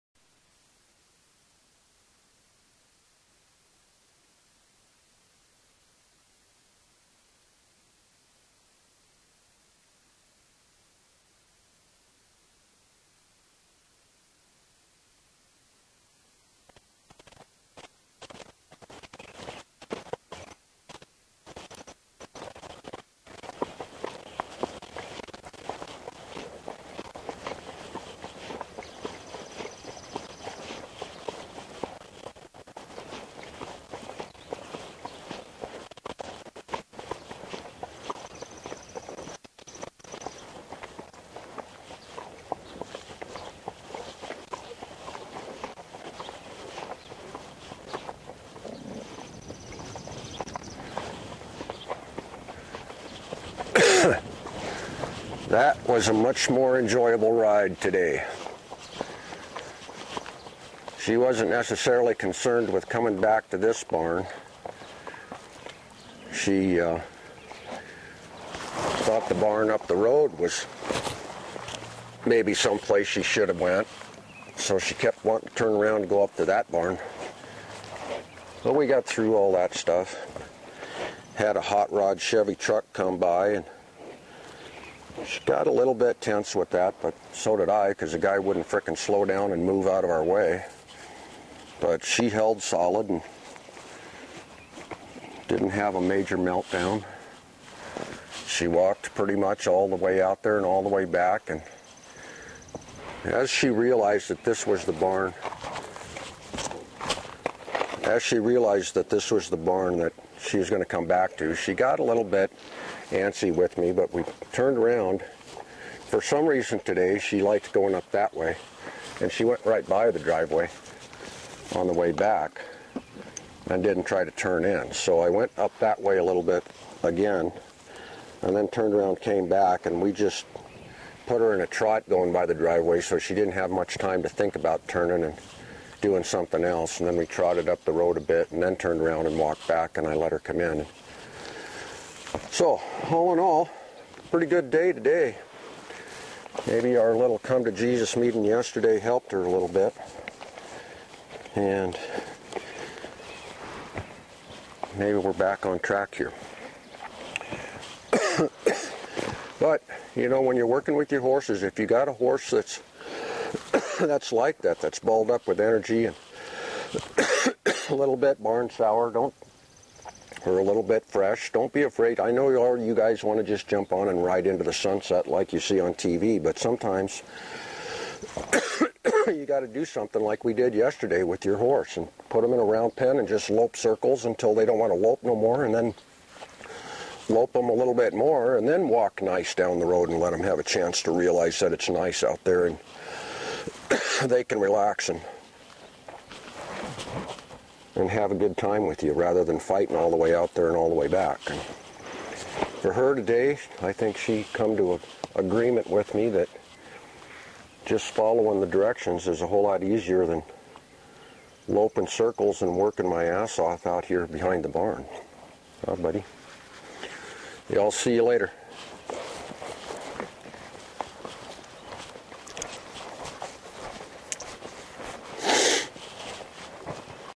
Indi D25 Finishing up a good ride with Smokie Brannaman
A good day had by all! Back on track with Indiana after yesterdays meeting of the minds!